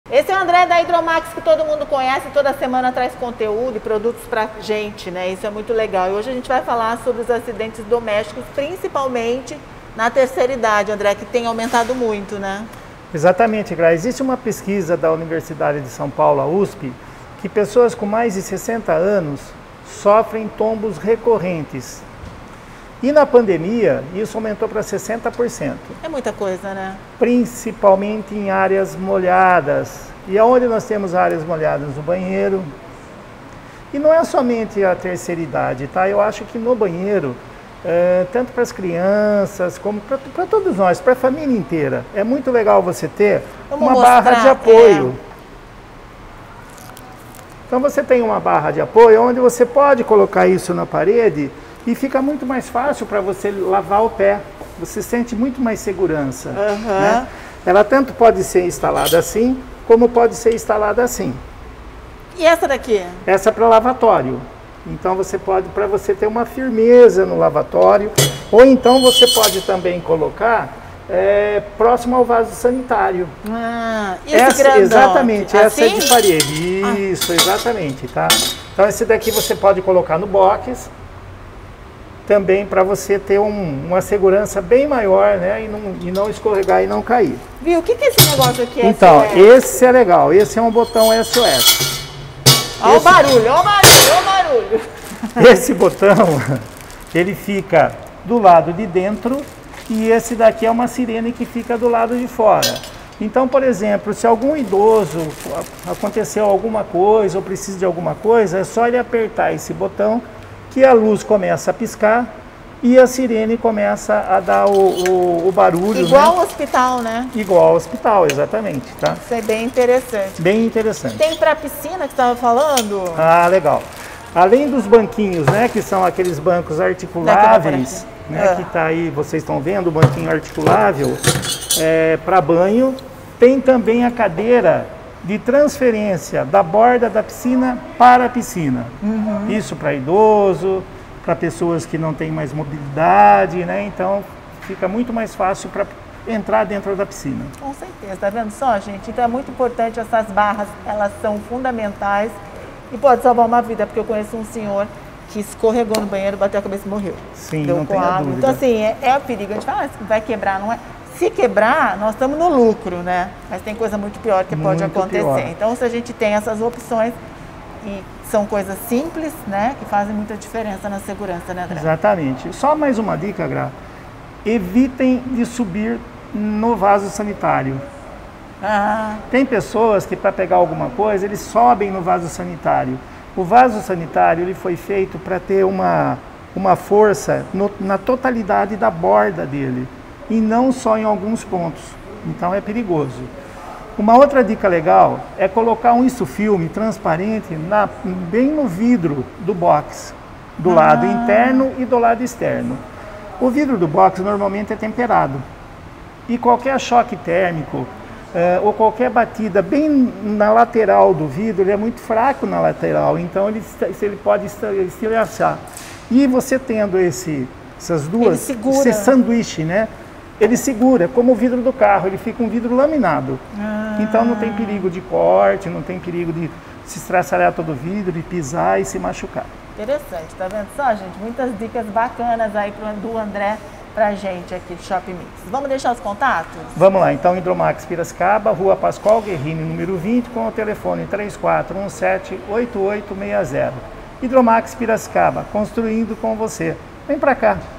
Hidromax Comércio de Materiais Hidráulicos
Hidromax sempre presente no nosso dia-a-dia para nos trazer mais qualidade de vida! Hoje é dia de mostrarmos um pouco as barras de apoio que vocês encontram na loja!

As barras de apoio são peças que auxiliam pessoas que necessitam de ajuda ao utilizar o banheiro, como idosos e portadores de paraplegia. Elas contribuem com a segurança e o conforto das pessoas no momento de utilizar o vaso sanitário, na hora do banho ou nos demais uso dos banheiros.